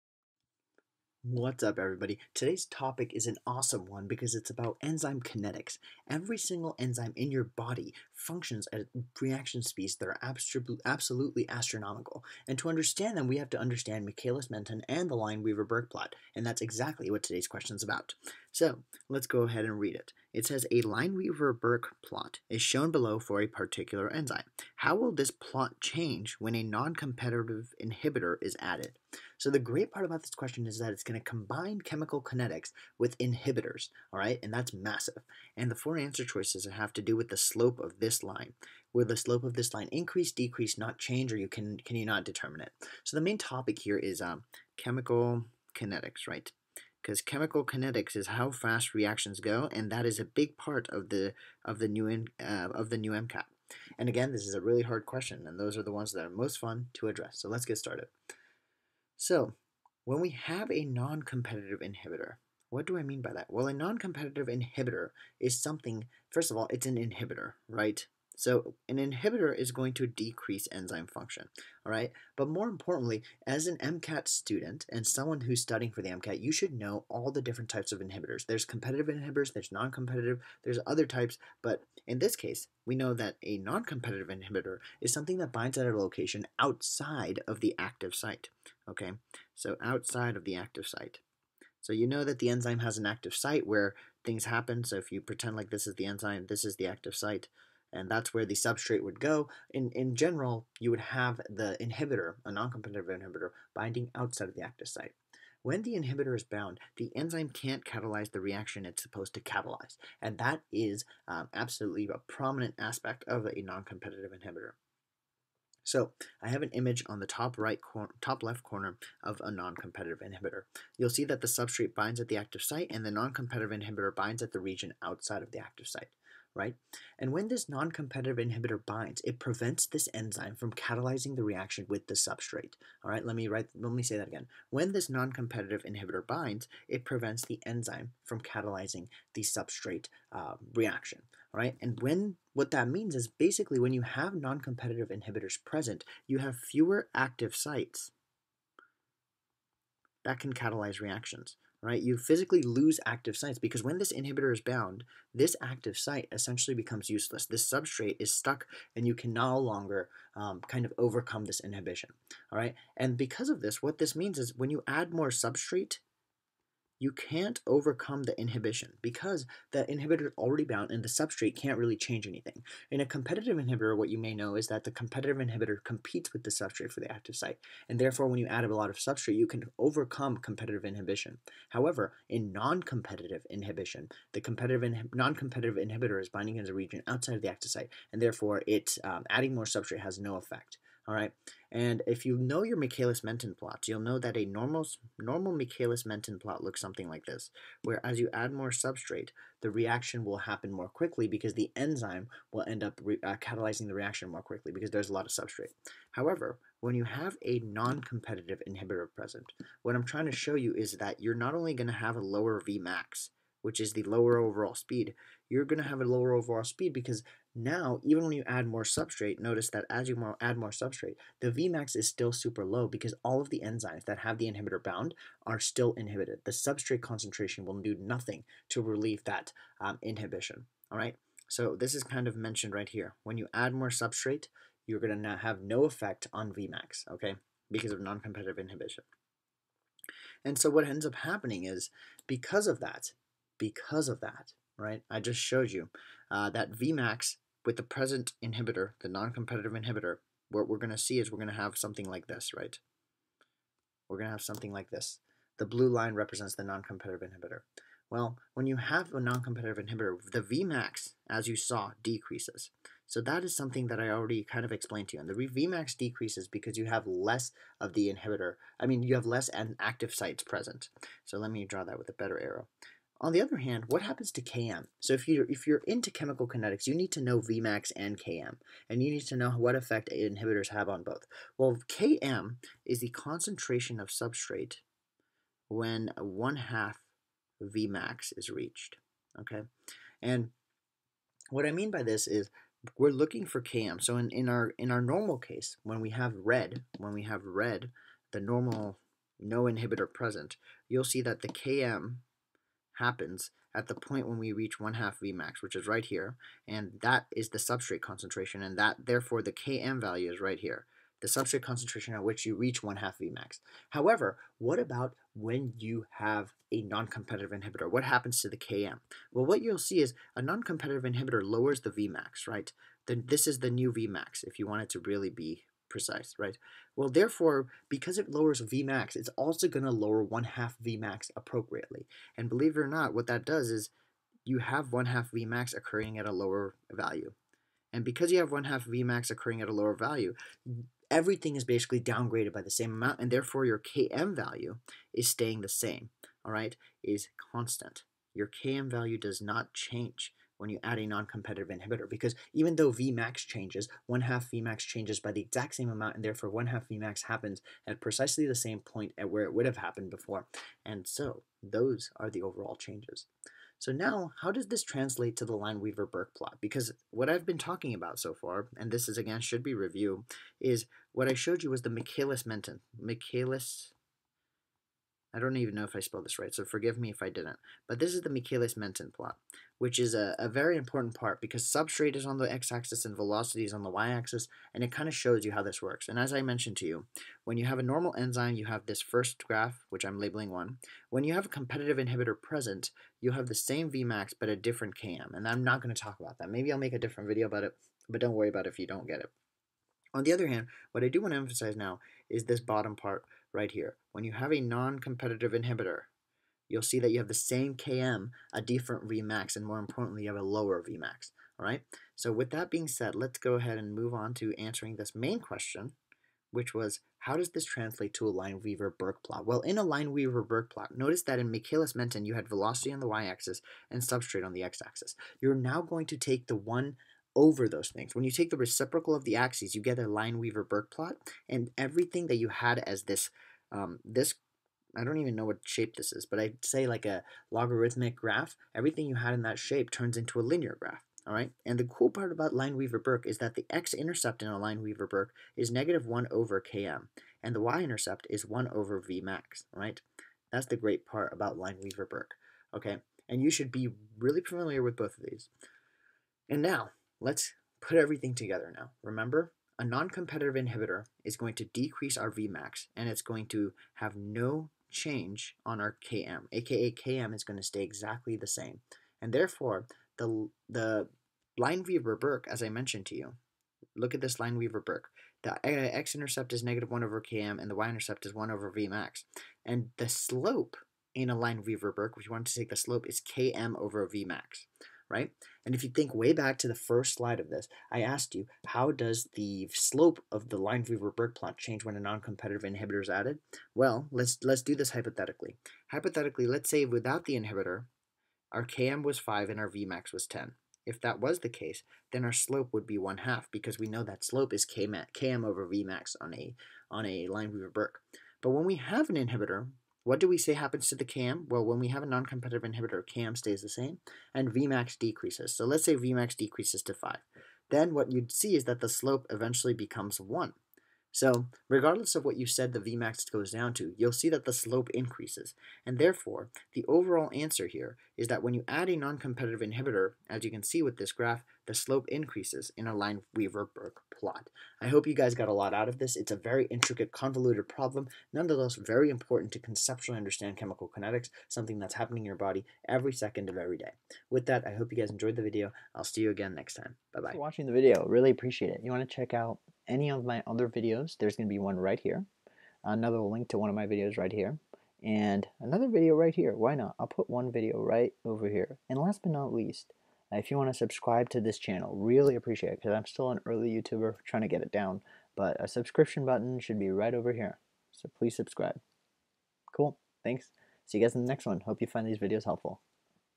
How Complicated Can Michaelis–Menten Kinetics Even Be?
The new MCAT has a huge emphasis on Michaelis–Menten kinetics, so I figured I'd go over a holistic question about it!